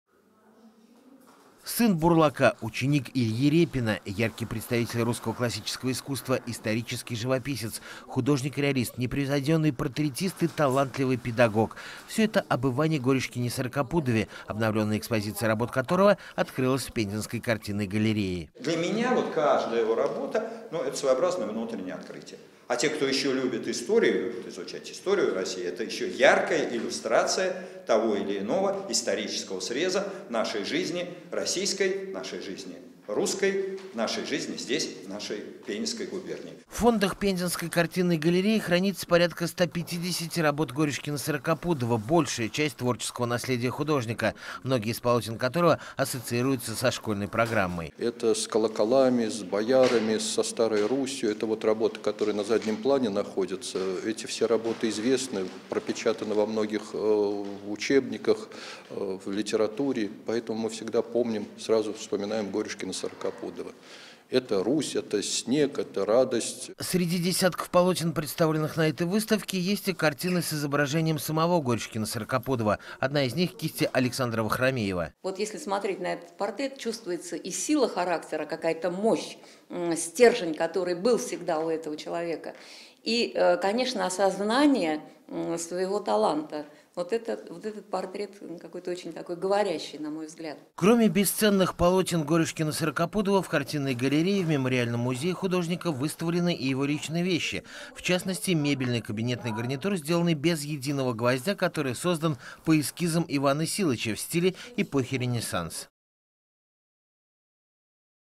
В Пензе открылась обновленная выставка Горюшкина Сорокопудова
В Пензенской картинной галерее открылась обновленная выставка Ивана Горюшкина-Сорокопудова. Иван Силыч - яркий представитель русского классического искусства, исторический живописец, художник-реалист, непревзойденный портретист и талантливый педагог.

«Для меня каждая его работа - внутреннее открытие. А для тех, кто любит историю, это еще и исторический срез жизни нашей губернии», - подчеркнул директор Пензенской картинной галереи им. К. А. Савицкого Кирилл Застрожный.
В фондах картинной галереи хранится порядка 150 работ Горюшкина-Сорокопудова, это большая часть творческого наследия художника. Некоторые из полотен ассоциируются со школьной программой.

«Картины с колоколами, боярами и старой Русью напечатаны во многих учебниках по литературе», - заметил коллекционер Александр Сычев.

Среди десятков работ, представленных на этой выставке, есть и изображения самого Горюшкина-Сорокопудова. Одна из них кисти Александра Вахрамеева.

«Чувствуются и сила характера, и осознание собственного таланта», - отметила заведующая экспозиционным отделом Пензенской картинной галереи им. К. А. Савицкого Нина Санталова.

Кроме бесценных полотен Горюшкина-Сорокопудова в картинной галерее, в мемориальном музее художника, выставлены и его личные вещи. В частности, мебельный кабинетный гарнитур, сделанный без единого гвоздя, который создан по эскизам Ивана Силыча в стиле эпохи Ренессанса.